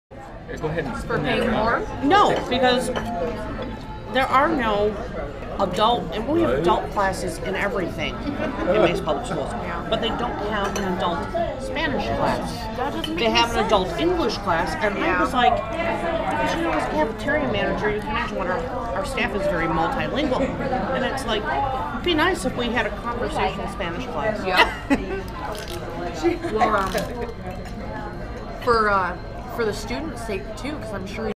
Greysen and Rachael Paetz's Baptism Dinner, Crackers & Co. Café, Mesa, AZ, 17 April 2016, GOPR0888
Greysen and Rachael Paetz's Baptism Dinner, Crackers & Co. Café, 1325 N Greenfield Rd, Mesa, Arizona, 17 April 2016, GOPR0888